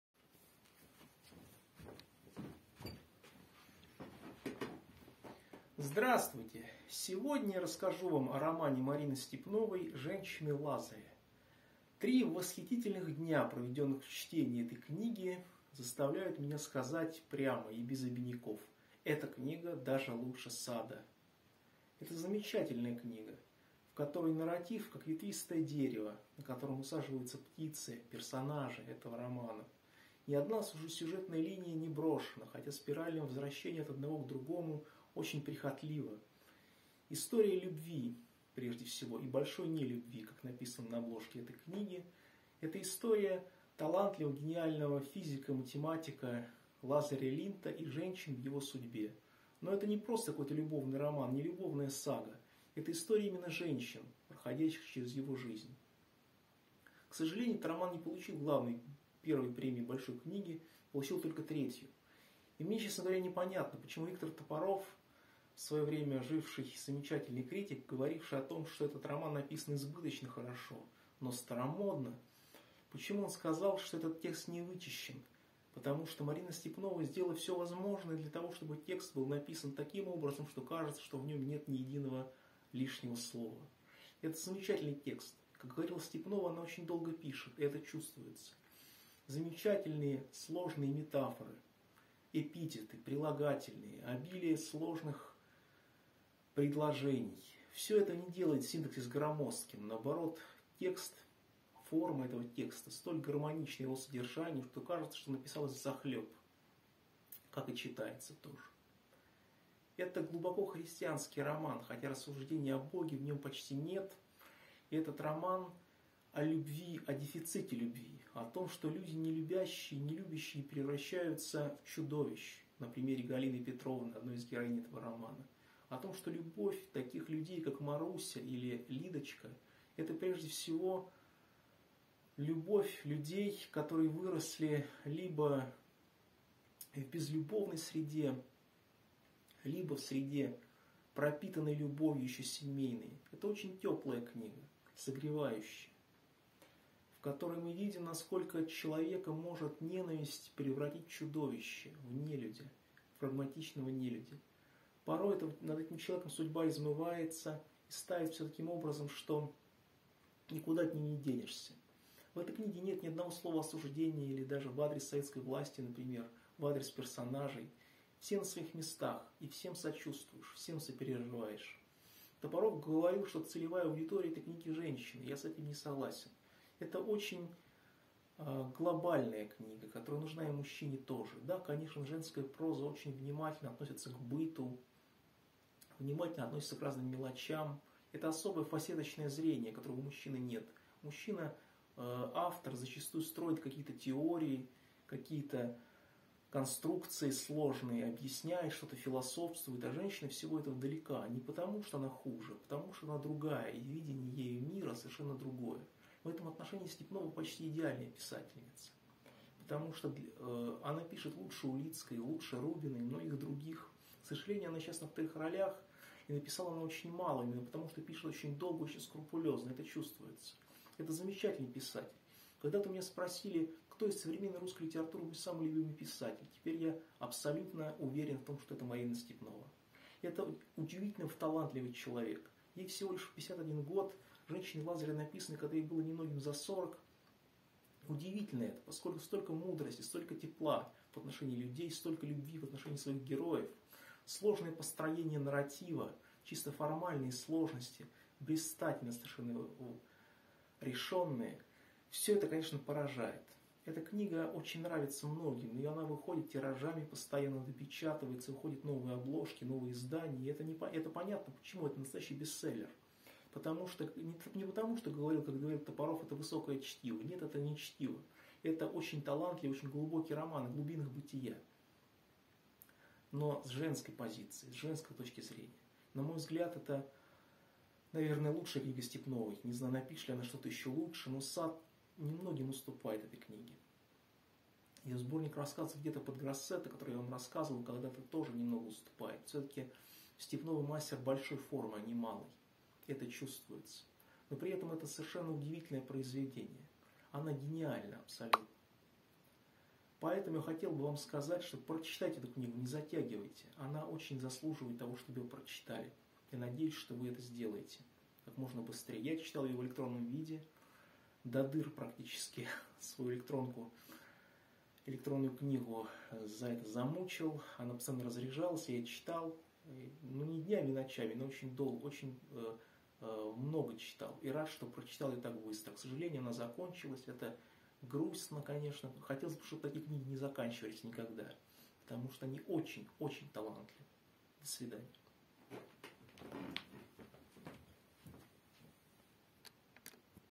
Марина Степнова "Женщины Лазаря"
Выпуск видеоблога посвящен обзору романа Марины Степновой "Женщины Лазаря".